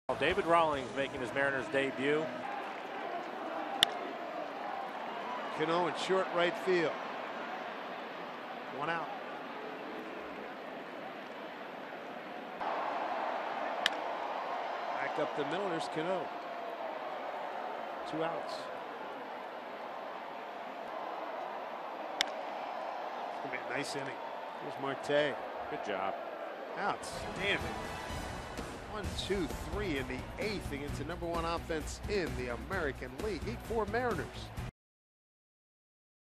SEA@BOS: Rollins tosses perfect 8th in Mariners debut
David Rollins makes his Mariners debut and pitches a perfect inning, throwing just seven pitches to retire the Red Sox